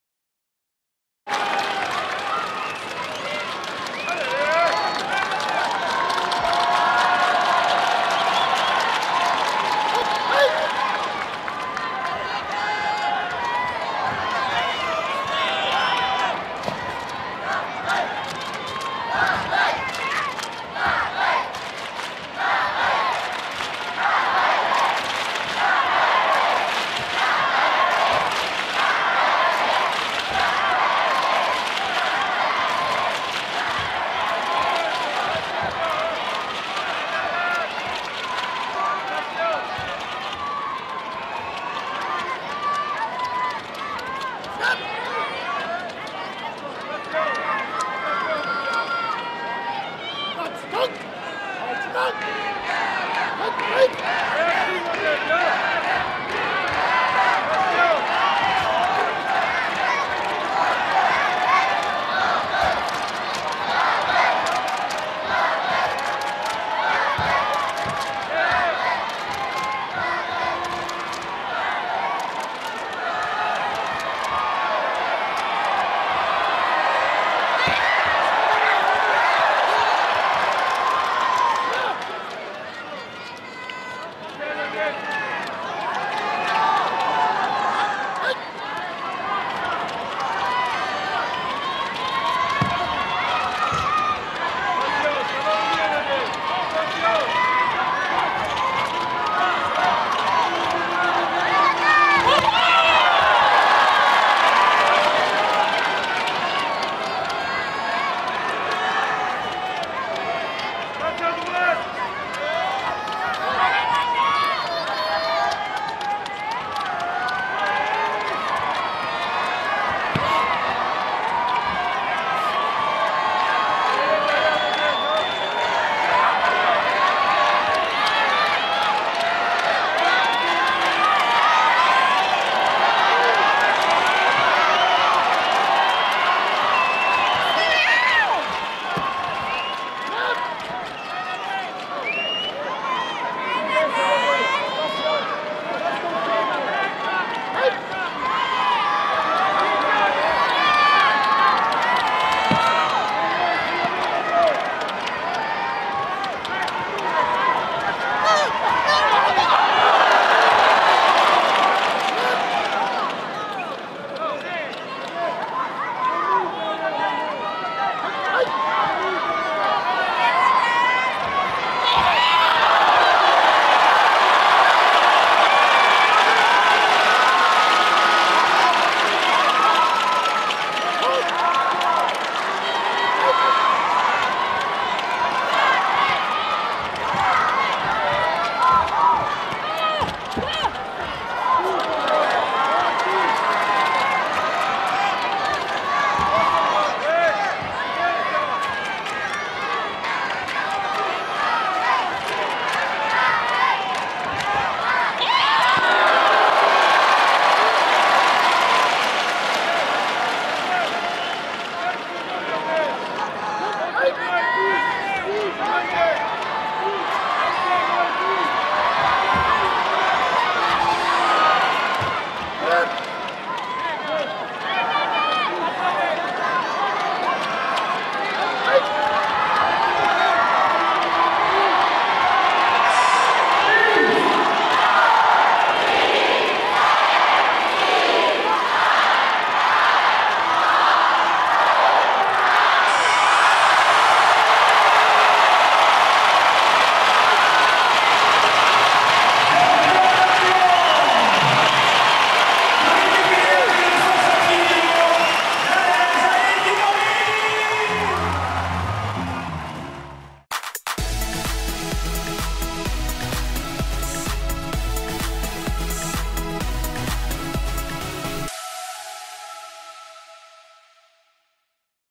グレタ・ヴィテッリ VS ナデジュ・アイット・イブラヒム【Greta Vitelli vs Nadege Ait Ibrahim】
2012年世界空手道選手権　女子+68kg級決勝戦【2012 World Karate Championships Women +68kg Final】

世界各地で行われている空手の試合動画です。
This is a video of karate games being held all over the world.